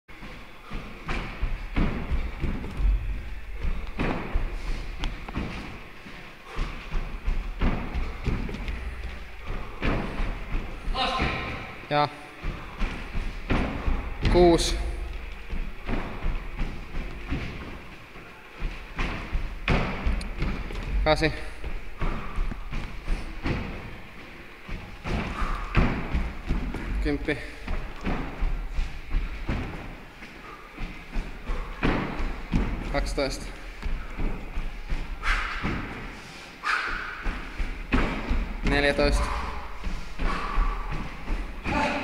Ville Sistonen inspirational court sprints
Traditional 3x20 court sprint session in preparation for the first tournament of 2016 in Lahti.
Due to technical difficulties only 15 sprints can be seen on this video.